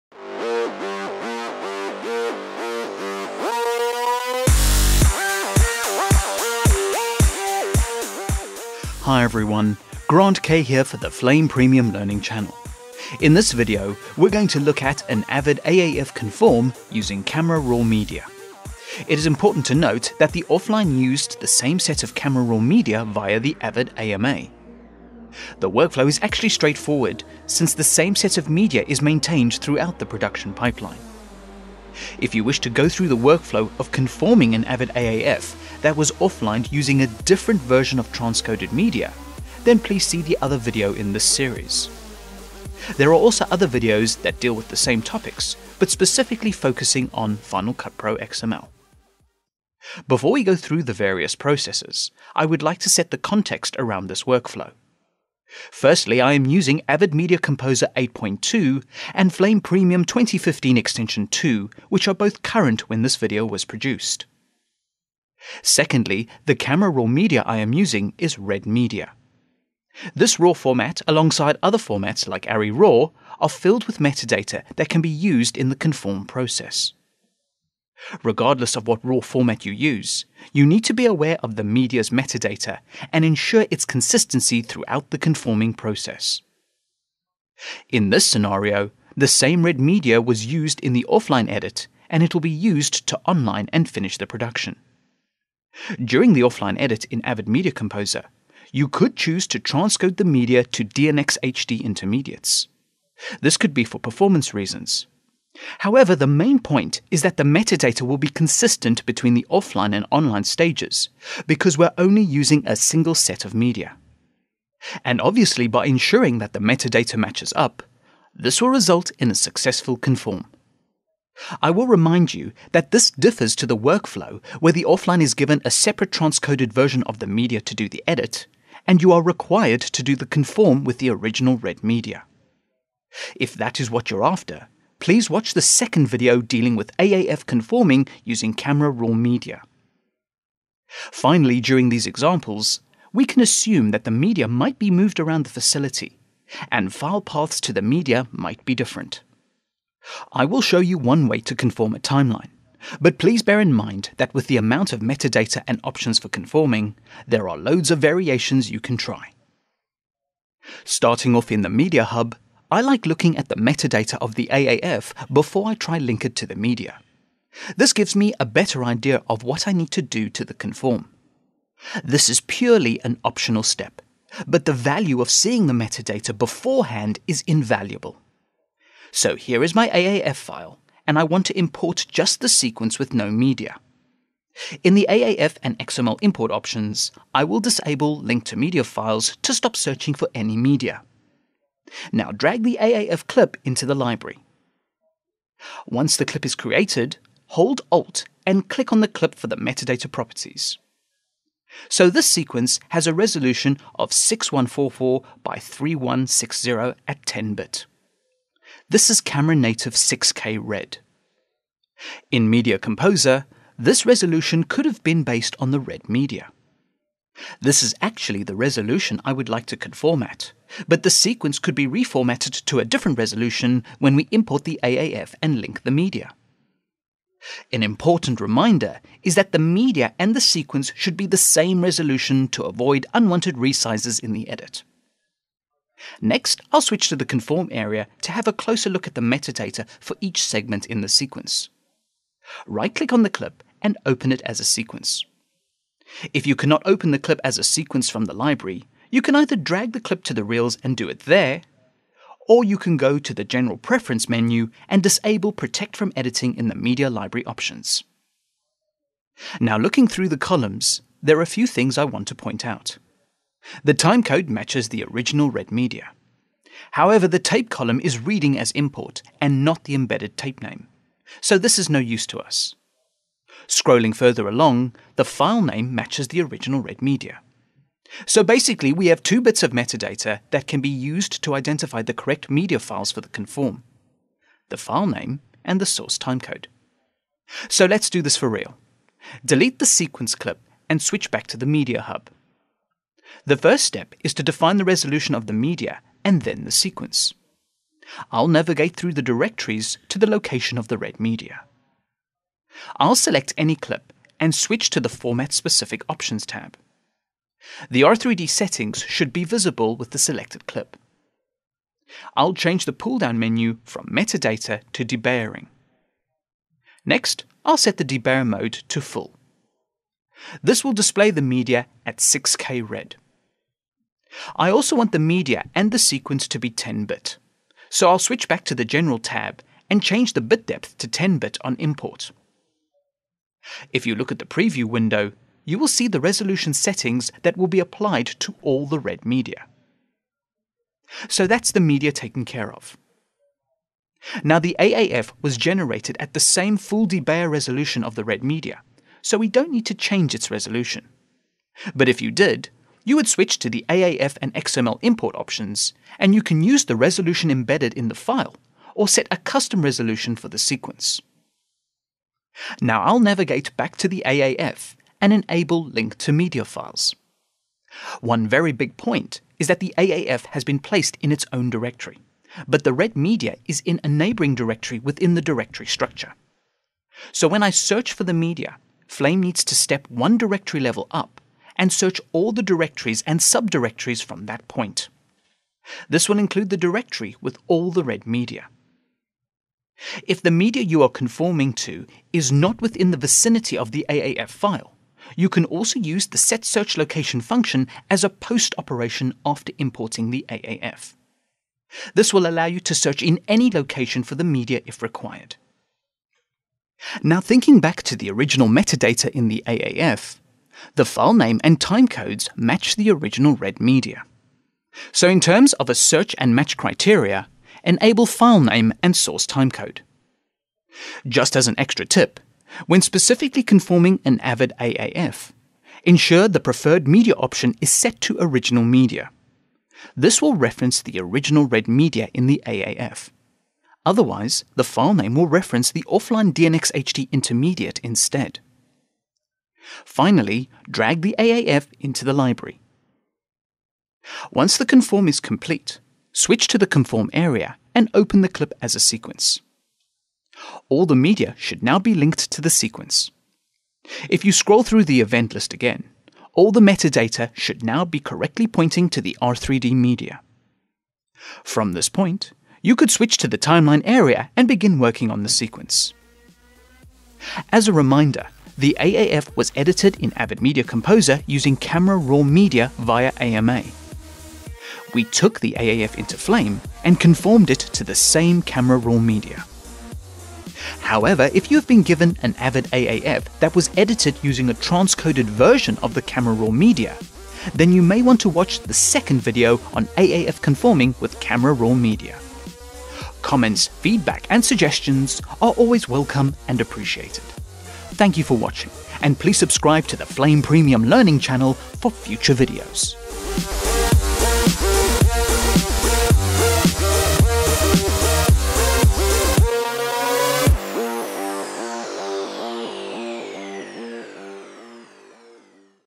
Conforming AAF using Camera RAW Media - Workflow 1
Level: Beginner
Recorded in: Autodesk Flame Premium 2015 EXT 2

In this video, We look at the conforming scenario where the Avid offline and Flame online stages of production use the same version of media throughout the production pipeline.

This workflow is applicable to RAW formats such as RED, ARRIRAW, etc.